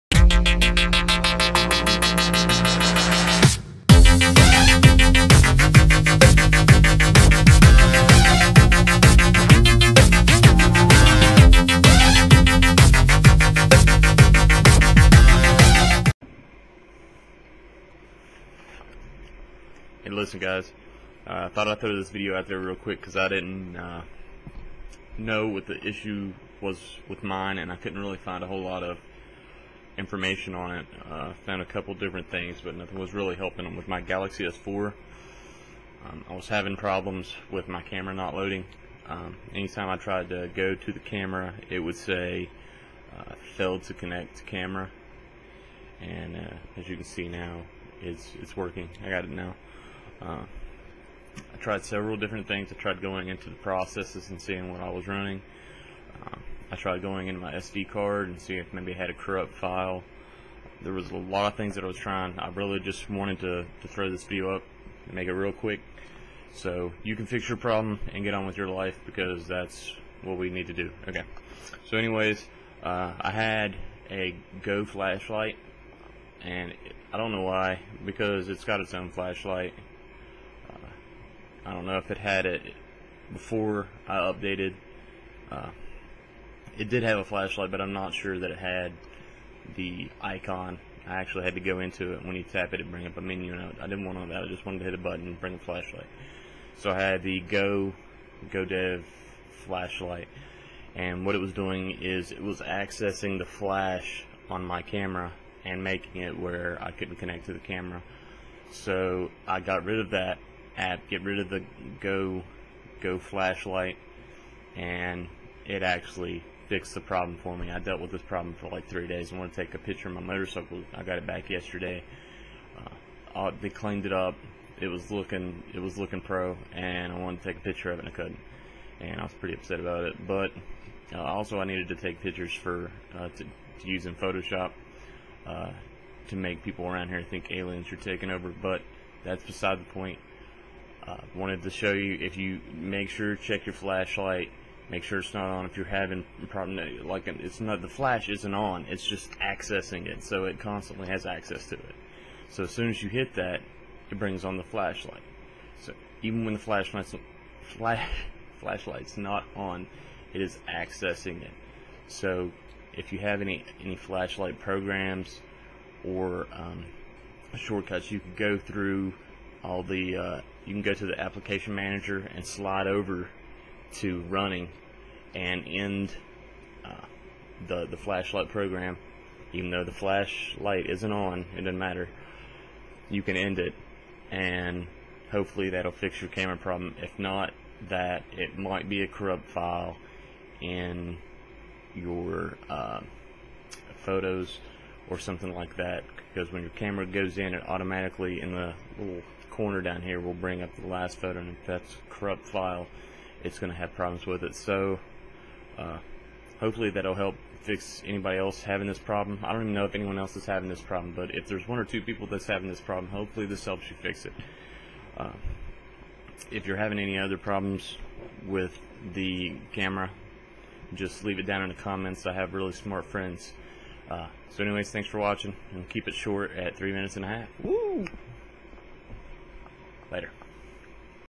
How To Fix "Galaxy S4" "Camera Not Working" - Easy Fix
Just a quick tutorial that hopefully will help you fix any problems you might be having with your S4 Camera. This fix can also work among other android devices with similar problems.